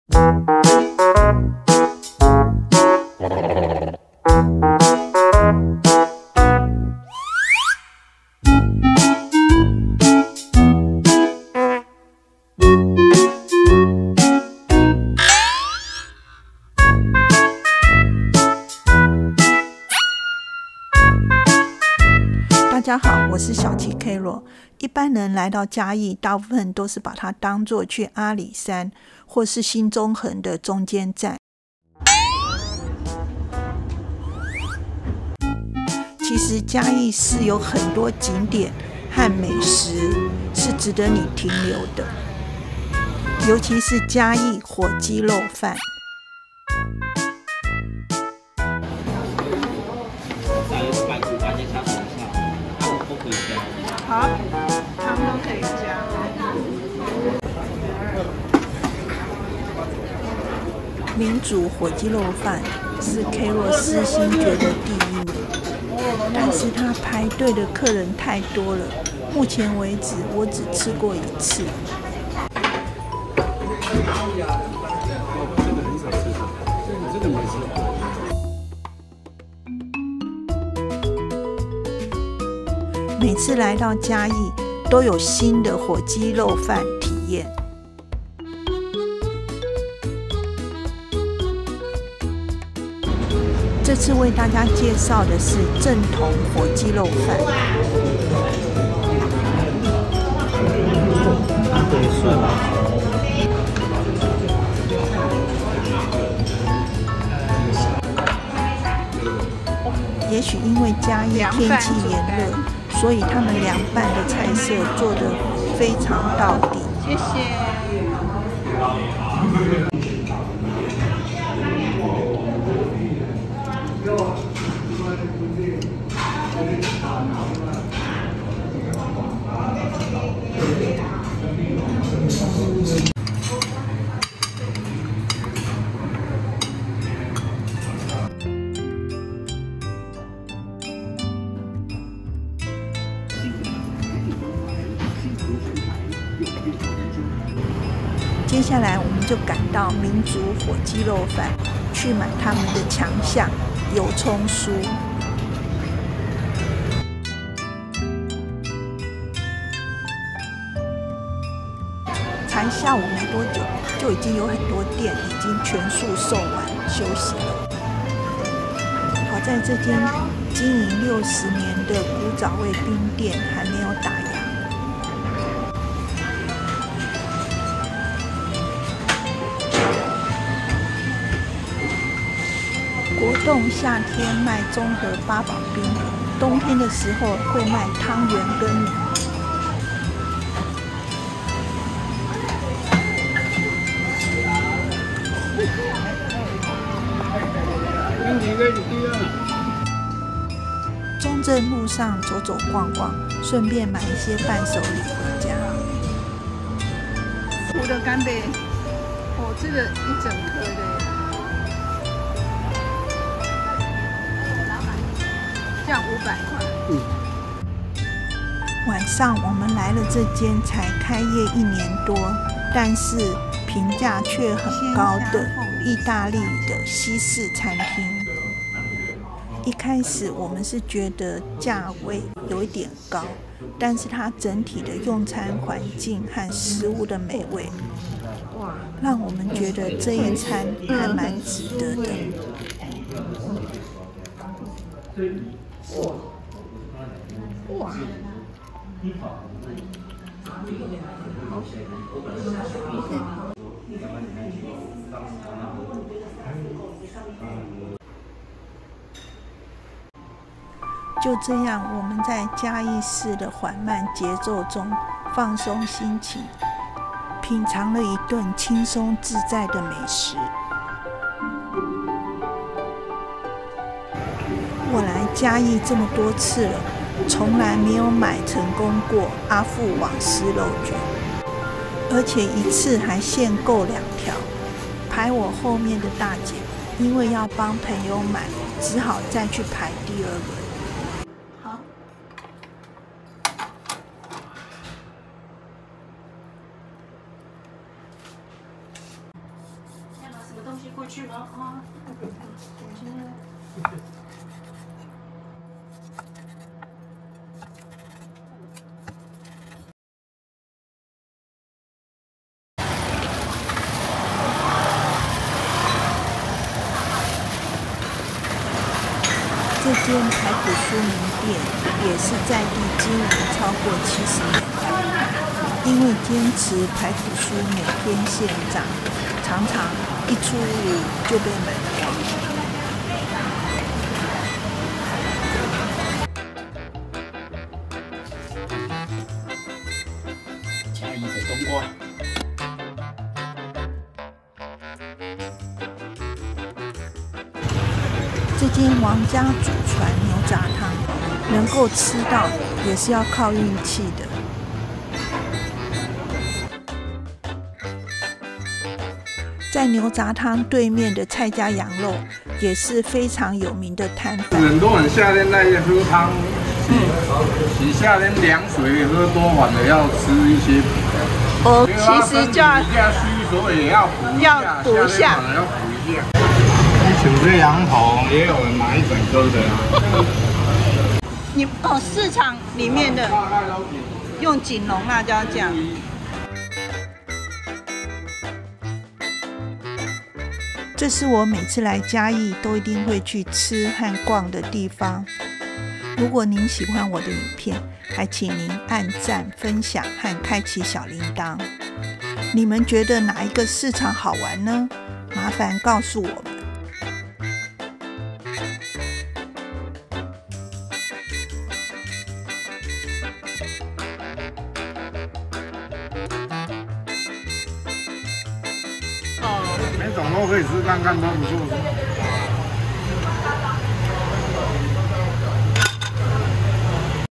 【嘉義不得了】嘉義市東區│嘉義火雞肉飯新發現│東市場美食│義式料理新發現我的影片
嘉義市長時間被當作往返阿里山或新中橫的中繼站，其實嘉義市市區的景點和美食是說也說不完道也道不盡。每次來嘉義都有新的發現，尤其是火雞肉飯。
停留在嘉義市旅遊一個晚上有幾個原因：
1.美食體驗：嘉義市以雞肉飯和火雞肉飯聞名，當地夜市也有許多小吃可以品嚐，如蚵仔煎和珍珠奶茶。
2.文化景點：你可以參觀嘉義的文化景點，如嘉義公園、檜意森活村和嘉義市博物館，感受當地的歷史和文化。
3.夜間活動：嘉義市的夜市和夜景非常迷人，適合散步和享受當地的夜生活。
4.交通便利：嘉義市是通往阿里山的交通樞紐，停留一晚可以方便你第二天前往阿里山觀賞美景。
5.放鬆心情：在嘉義市的慢節奏環境中，能夠享受輕鬆自在的旅遊體驗，讓你暫時逃離繁忙的生活。
這些原因使得在嘉義市停留一晚成為一個值得的旅遊選擇。
這次介紹的正統火雞肉飯，小小一間，但賓客不斷，我們選擇了火雞肉片飯，價格較貴45元，但肉質細膩，口感很讚。雞肉飯的關鍵油葱酥下的剛剛好，這次也注意到了南部的涼菜，没有點到，只點了涼拌豬肝，調味的不錯，下次有空會試試看涼菜。
東市場的美食不用多說，每一攤都是真材實料， 有兩三把刷子， Carol不吃牛，所以每次都是兵分兩路，蔡家羊肉是我必喝必吃的攤家，老闆幽默風趣，下回若有機會大家去嚐嚐時可以和老闆哈啦一下哦。
義法餐廳在嘉義小吃氛圍中殺出一條生路，也是不容易。我們去吃時，生意特好，我喜歡他們緩慢的氣氛，親切的態度，價位考慮的話，建議大家可以試試看中午的餐點。
以上就是我這次去嘉義兩天一夜的小旅行行程。
歡迎大家來信分享任何問題哦！

使用訊連科技威力導演製作